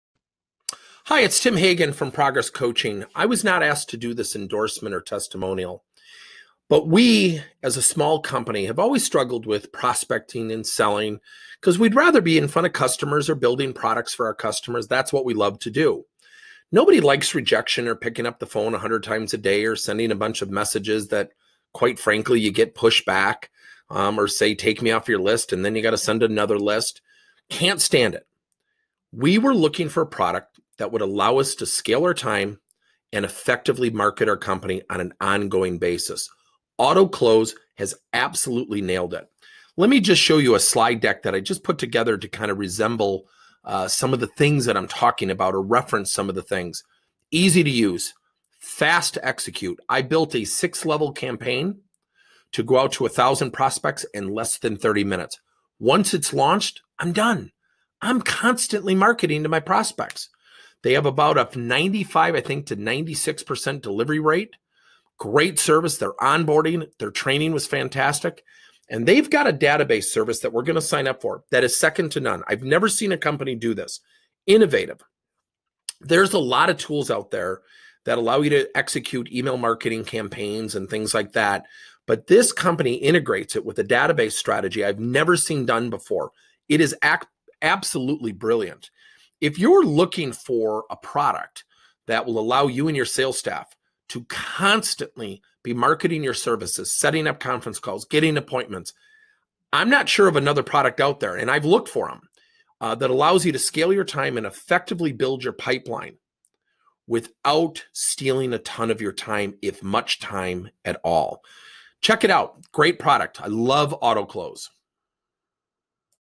Sales engagement platform that makes a difference
Autoklose Testimonial – Tim Hagen, President of Progress Coaching

Listen to one of our most amazing customers who took the time to record this video and share his experience with you.

The Future of Sales Automation! Autoklose is the last all-in-one email sales automation platform that you will ever need.

No credit card or commitments. Take 2 minutes and try it NOW